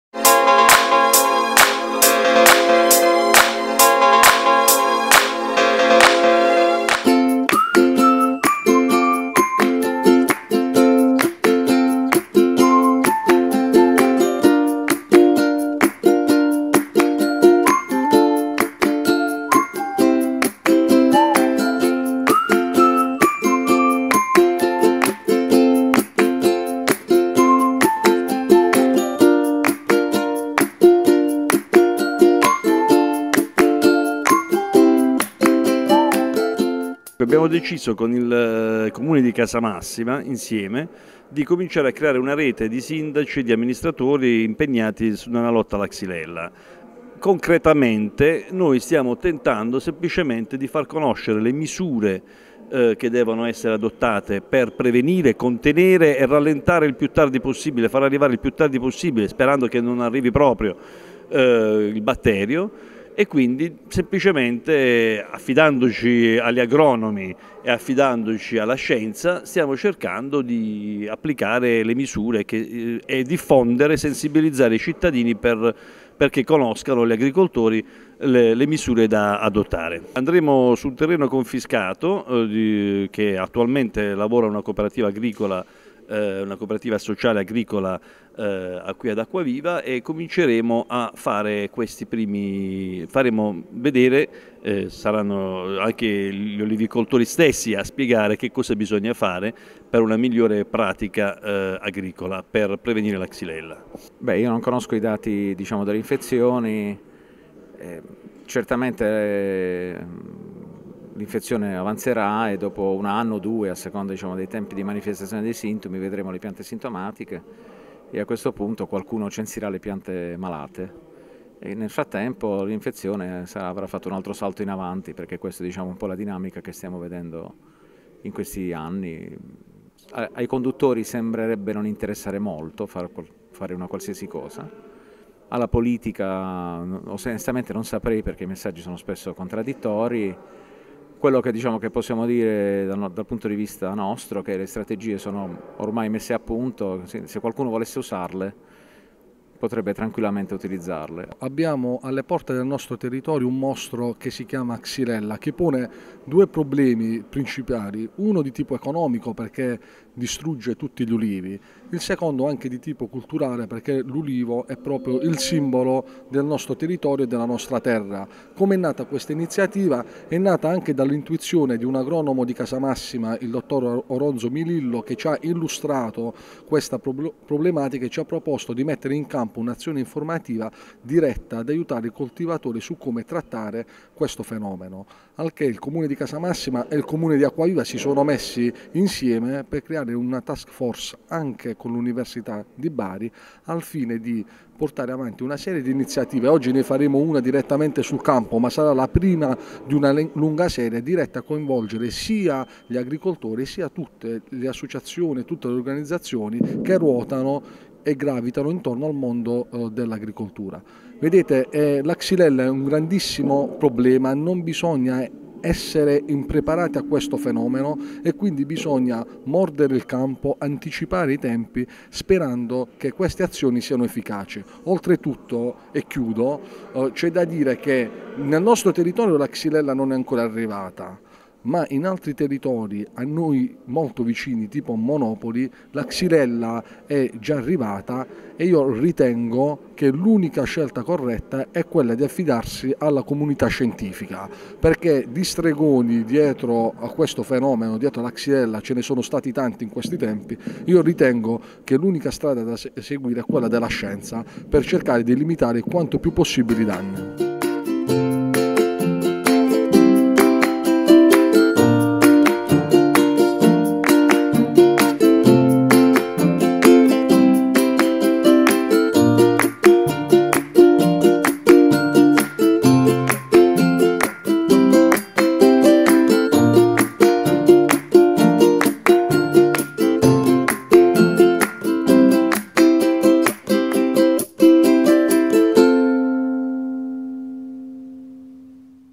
FOGLIE TV - Combattere la xylella: passiamo all'azione!
Dimostrazione pratica delle misure anti xylella, misure di contenimento per prevenire la diffusione del vettore: buone pratiche viste direttamente sul campo, potatura, aratura dei terreni agricoli e tutte le buone pratiche che sono presenti nell'avviso pubblico che il comune di Acquaviva delle Fonti ha pubblicato nel mese di gennaio. L'iniziativa, organizzata in sinergia dai comuni di Acquaviva delle Fonti e di Casamassima, è stata preceduta da un focus tenutosi presso l'oleificio sociale di Acquaviva; poi tutti in campo (in un terreno confiscato alla criminalità) per combattere la xylella.